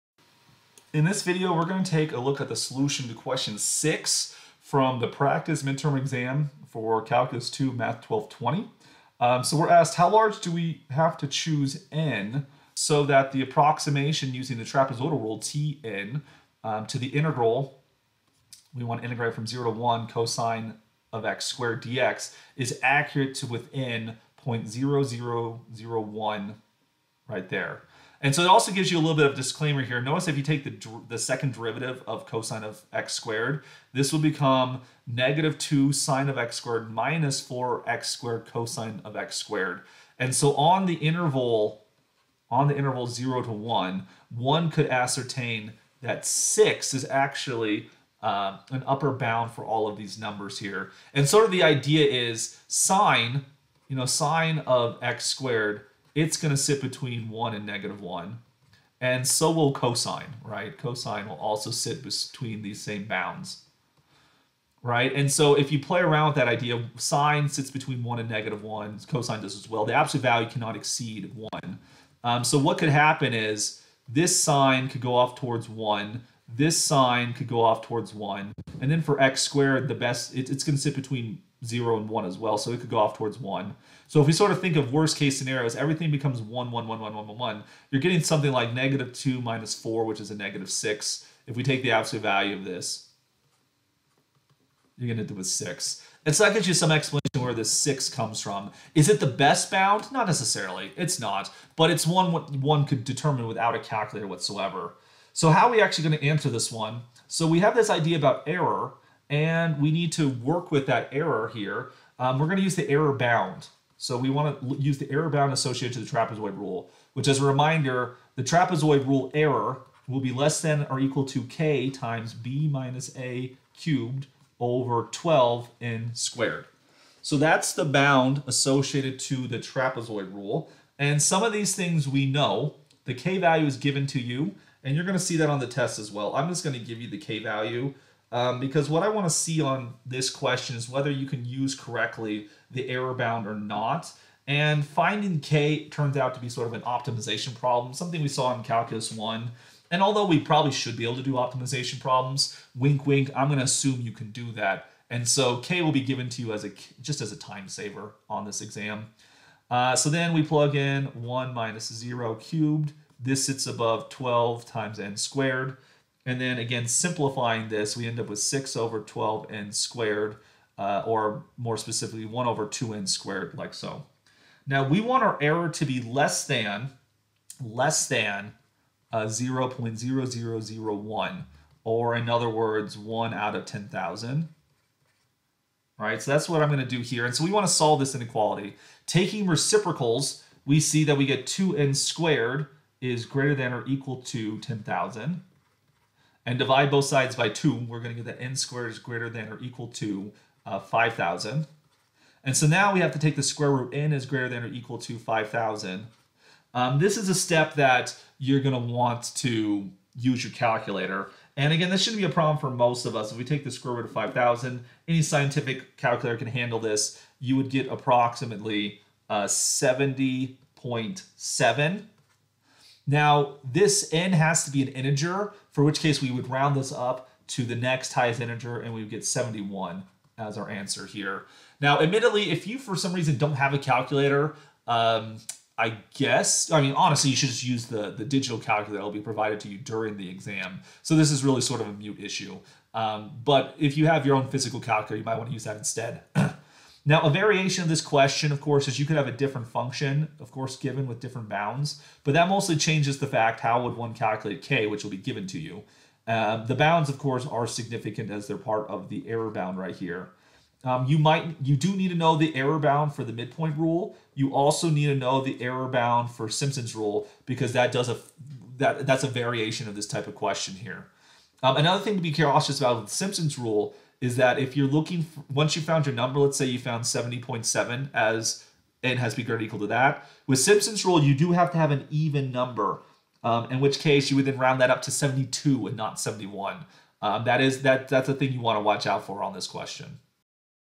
Midterm Exam, Math 1220, Question 6 (Integral Error)
We discuss how to determine the smallest possible number of subdivisions that guarantees that a trapezoidal approximation of a definite integral is within a specified error.